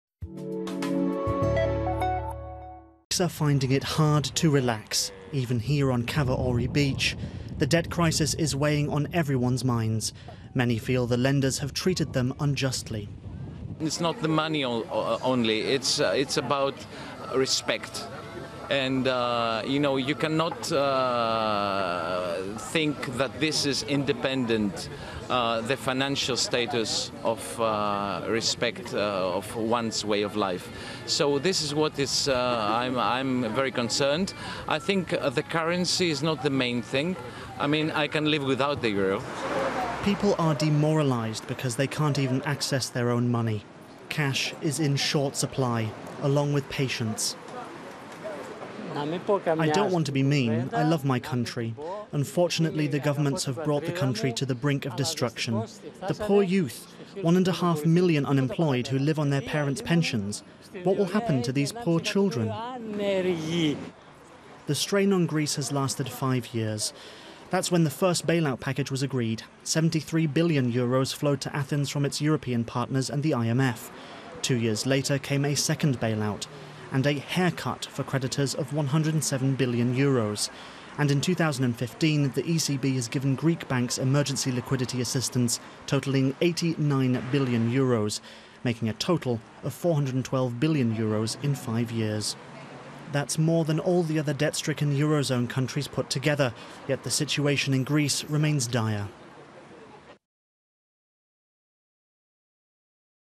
Greece: cash and patience in short supply | DW News
Ireland, Portugal and Spain have all left the bailout-phase behind. Only Greece sinks ever deeper into debt.
It has received more Emergency Liquidity Assistance than all other debt stricken eurozone countries put
together yet the situation remains dire.